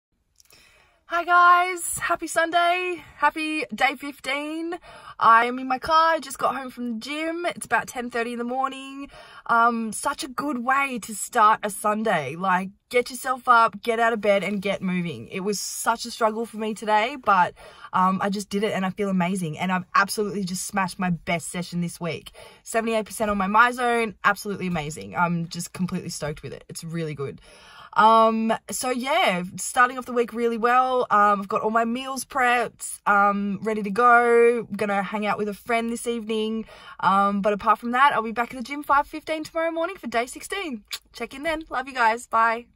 DAY15 21DAYFITNESSCHALLENGE
Just a quick update today, I'm really going to start making a conscious effort to think of more interesting things to say in days I only do a quick check in, especially when I'm going the same thing daily lol x